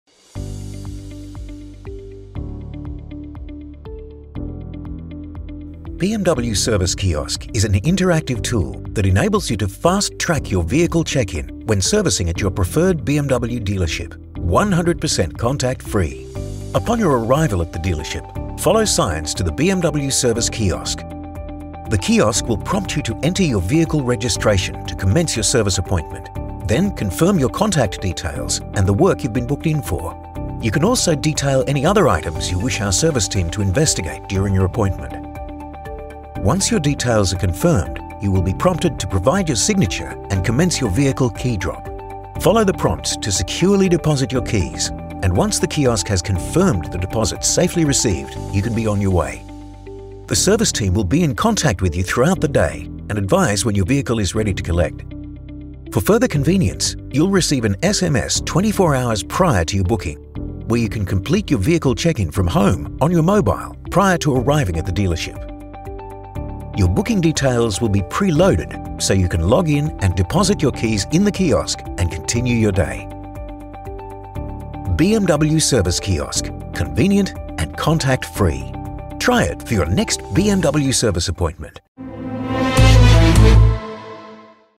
BMW Service Kiosk
BMW Online Service Check-In provides you with the flexibility, freedom and security to drop off your BMW for your next service appointment completely contact-free. Experience it for yourself at your next BMW Service.